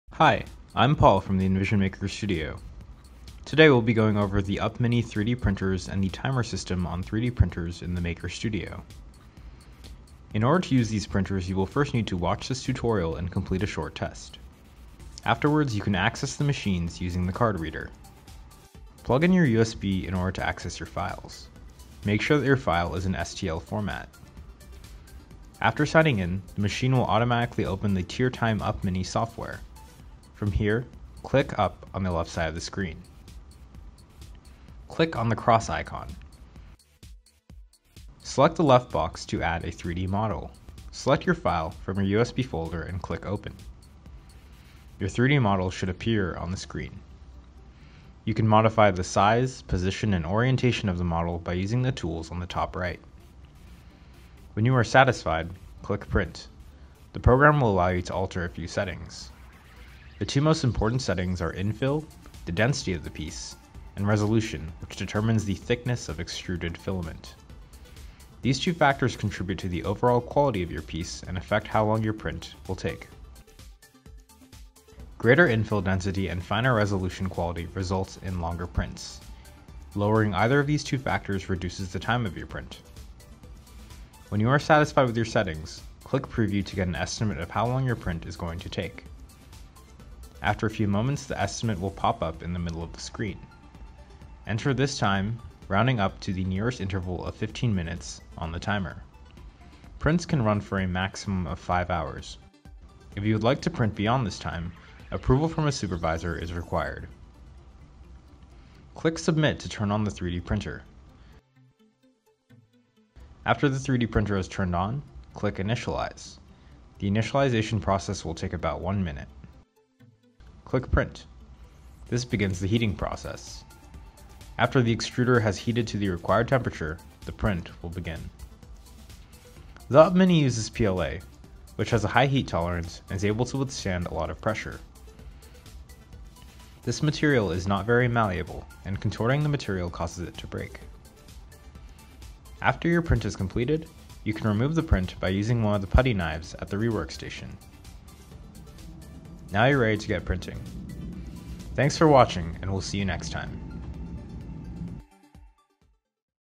EnVision UP-Mini Tutorial
Watch this video for an introduction to the UP-Mini 3d printer and the EnVision Tutorial System.